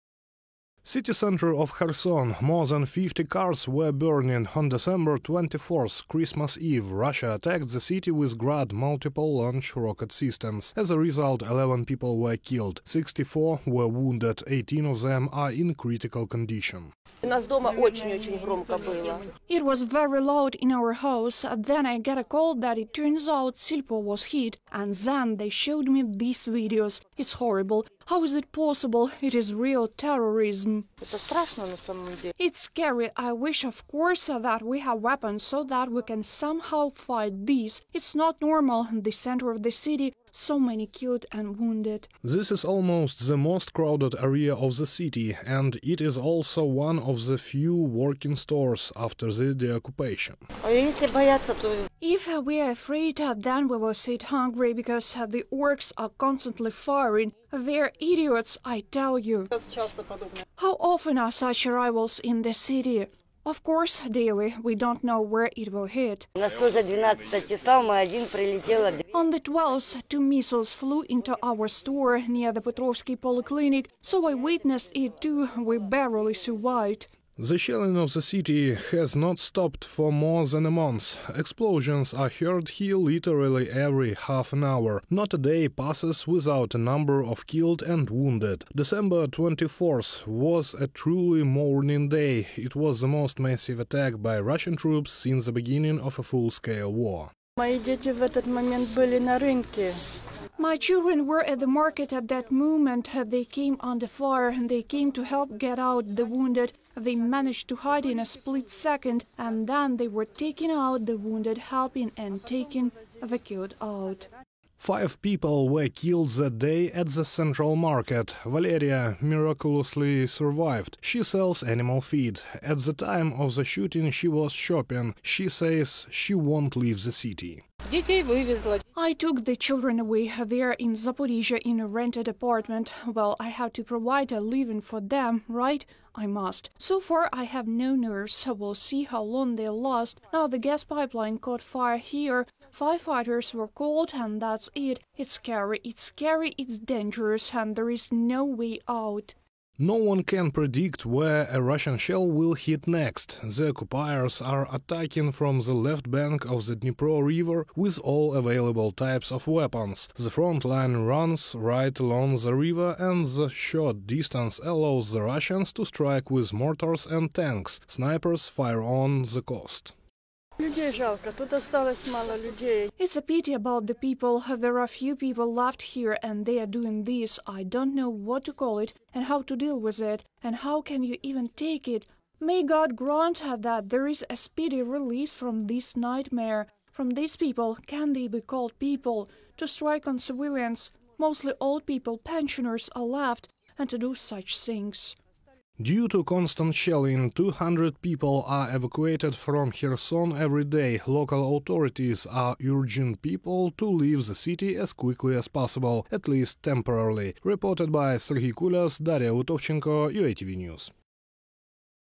Kherson under multiple shelling has Russian continue watch 🇷🇺
russian ukraine kherson millise rain down on Kherson has Russia advence watch 🇷🇺 lastest update on Kherson people on ground Tell it at !